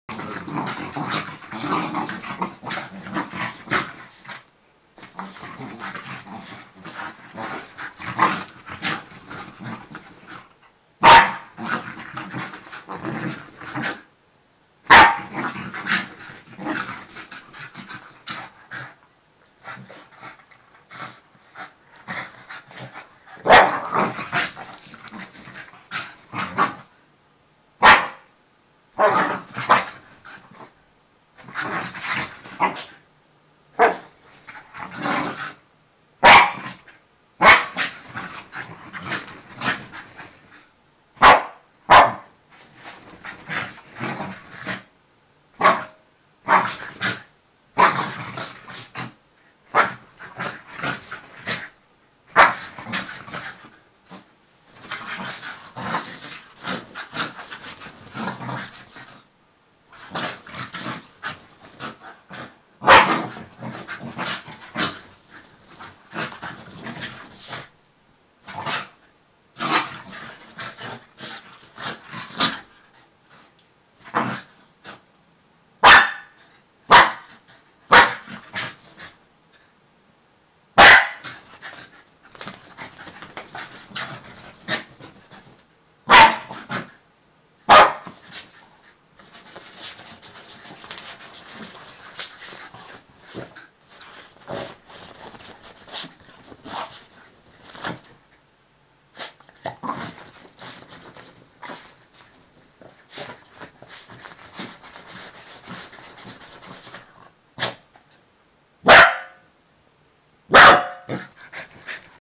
Our scotties Welcome home -ritual
They go just crazy everytime I come back from work :D Sorry about the blurriness in the beginning. New phone :/

Joka kerta kun tuun kotiin töistä, ni tää on mua vastassa :D Video on hiukan epätarkka alussa, anteeksi siitä. Uus puhelin millä kuvasin :/